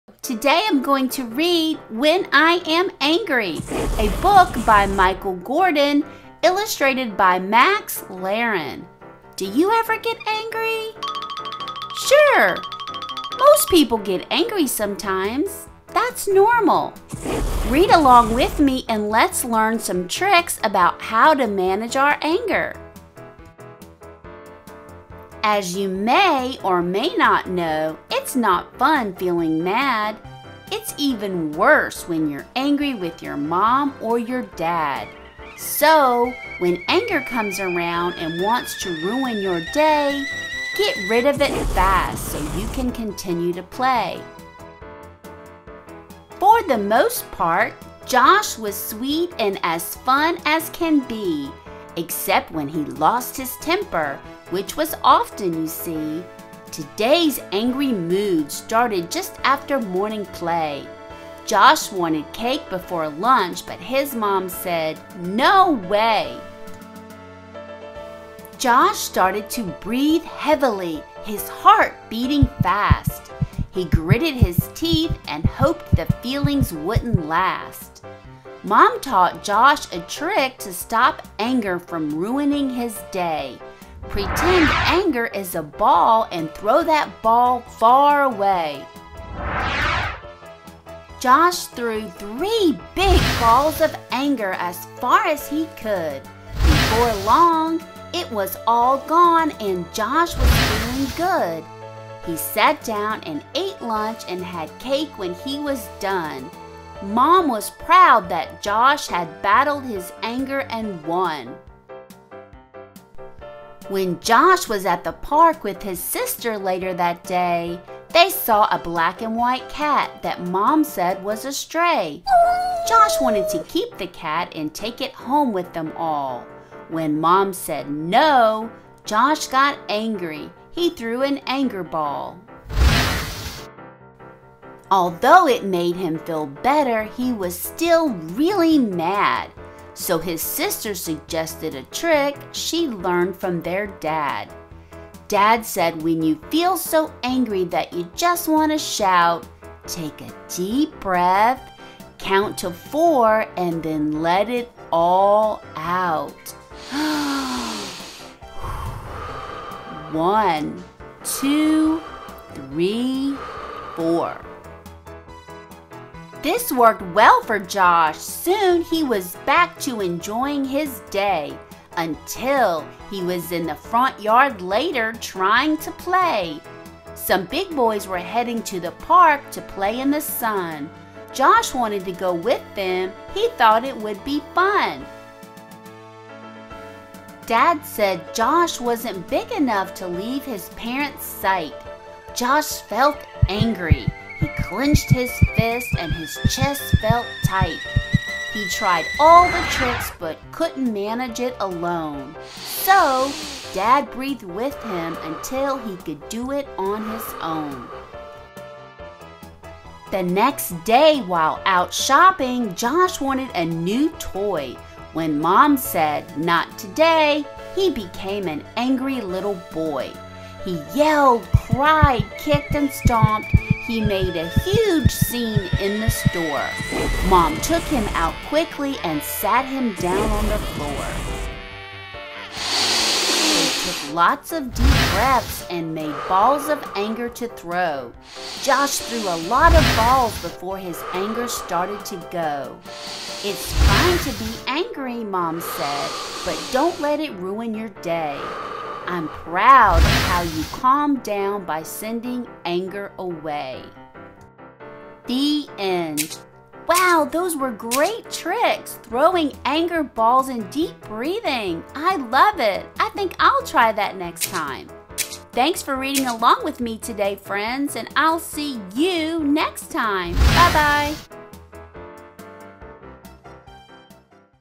📚 Kids Book Read Aloud: "When I Am Angry" by Michael Gordon ⭐
When I Am Angry is a children's book that teaches about bad tempers, tantrums and anger management for kids.  It has a cute story with a few tricks you can learn to calm down after getting upset.  Sunny Star reads the story book out loud and we add graphics and sound effects to make the kids book come to life.

Did you know that anger isn't all bad?

* Teach children to identify their anger signs
* Anger is a normal, healthy emotion.
* Teaching your kids to deal with disappointment
* Solutions that help kids handle their hot feelings in healthier ways

Sunny Star ⭐